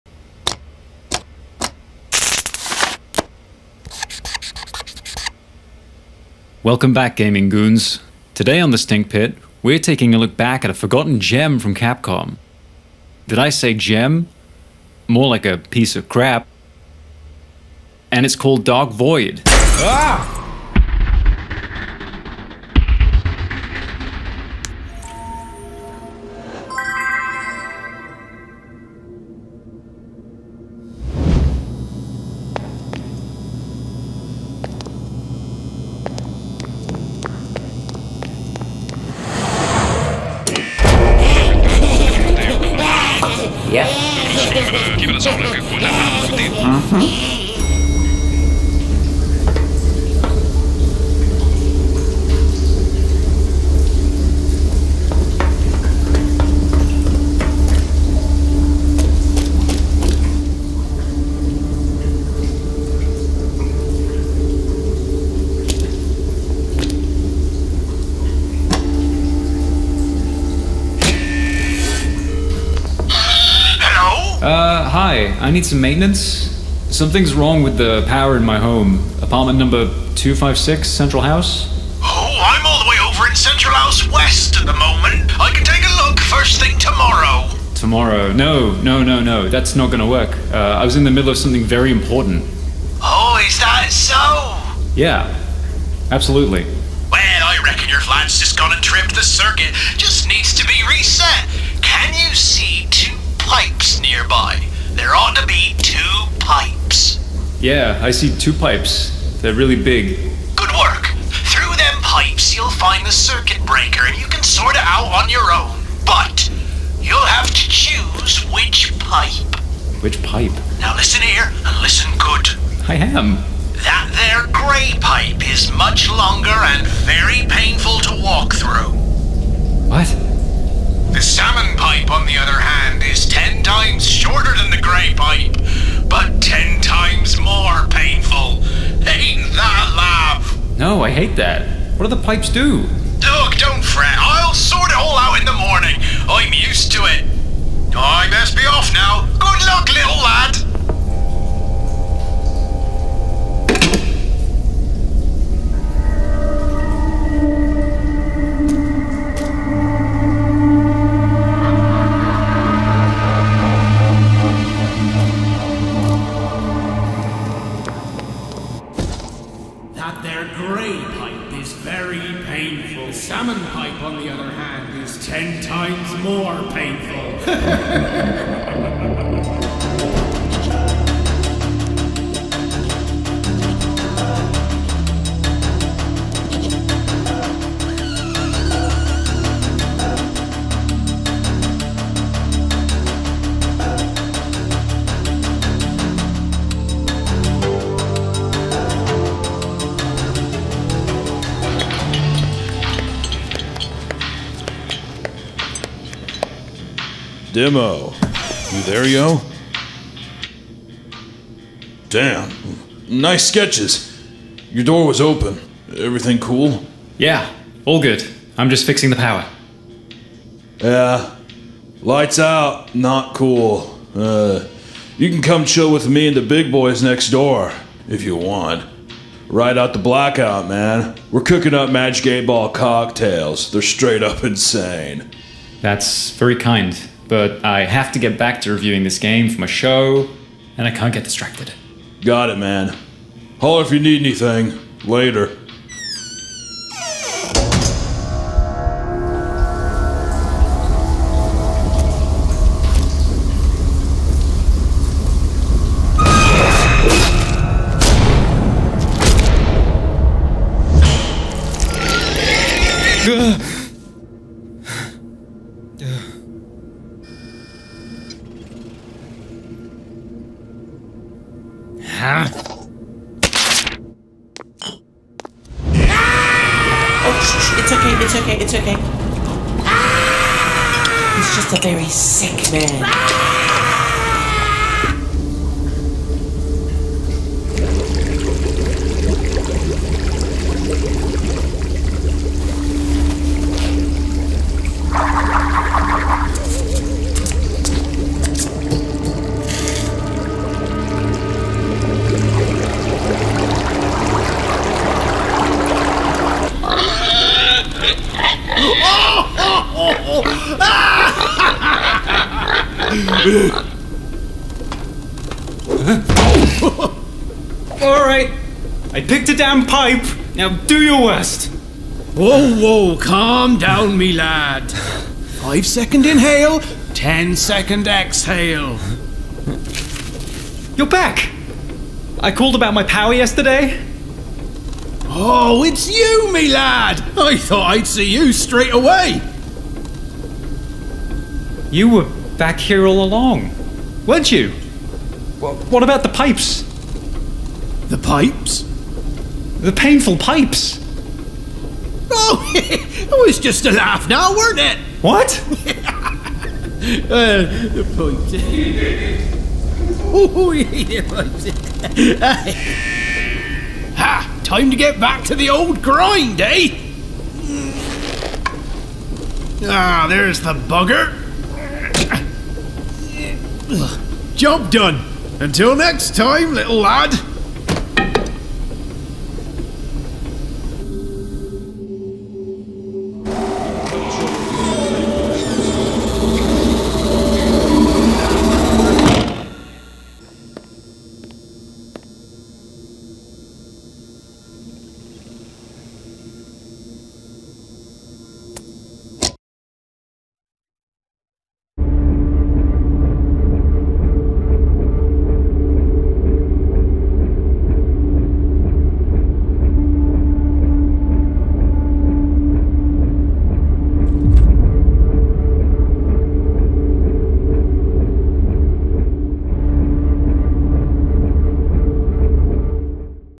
TWO PIPES
TWO PIPES - Demmo descends deep into his building's maintenance level in order to restore power to his aprtment. Will he ever finish his Dark Void for Playstation 3 video review?

*The poster sale has ended. Thanks for checking them out!

Cast
@hoolopee as Demmo
@HarryPartridge  as Maintenance Mark
@MeatCanyon as Beady
@psychicpebbles   as Grim Baby
@CusackCreatures  as Grim Mommy